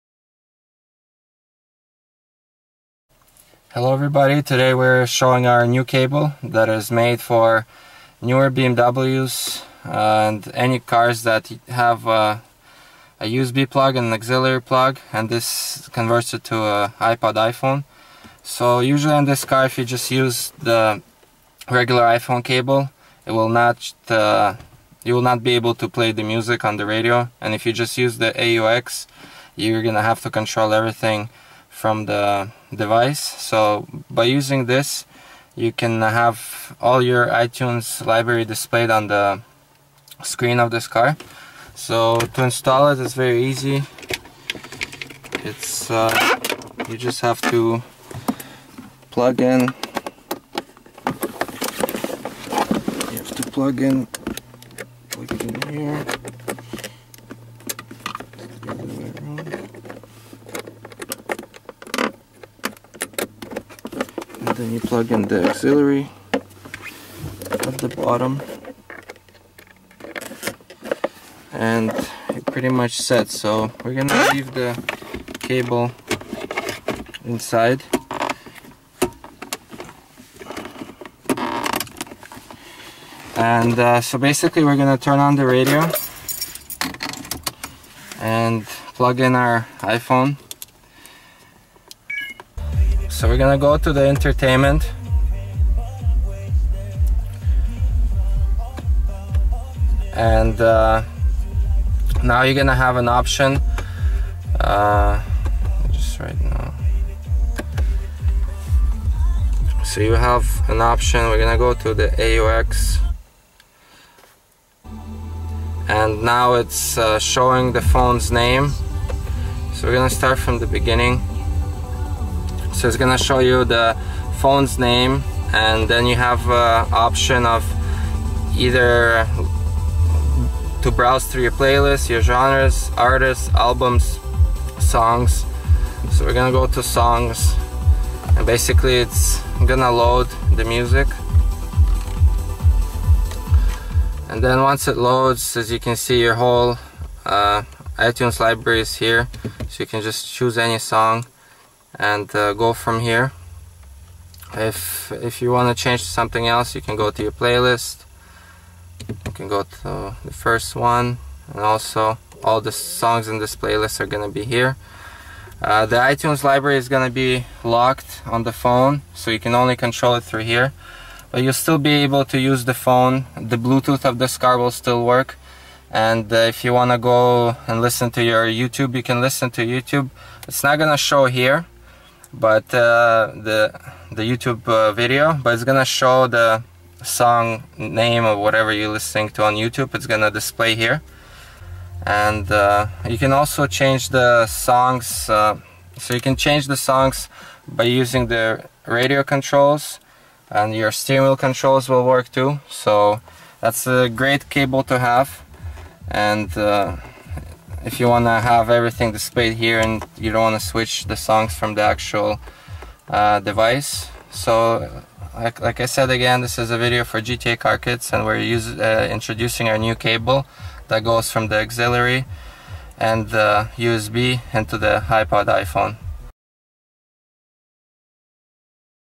GTA Car Kits - BMW cable from AUX/USB to iPod/iPhone/iPad (in E60)
- Adapter cable to connect iPod or iPhone to the audio system for the display of tracks and playlists.
Your iPod or iPhone is charged via the adapter cable as you drive and can be controlled either by the multifunction steering wheel, the iDrive Controller or the radio.
iPad can be controlled but cannot be charged.

Compatibility with the following BMW models•1 Series
•3 Series Sedan from 01/2012
•3 Series Sedan from 03/2005
•3 Series Touring from 09/2005
•3 Series Coupe from 09/2006
•3 Series Cabrio from 03/2007
•5 Series Sedan up to 03/2010
•5 Series Touring up to 09/2010
•5 Series Gran Turismo
•6 Series
•7 Series from 11/2008
•X1
•X3 from 1/2011
•X5 from 03/2007
•Z4 Roadster from 02/2009

The video was filmed in BMW 5 series 2008 (E60 style)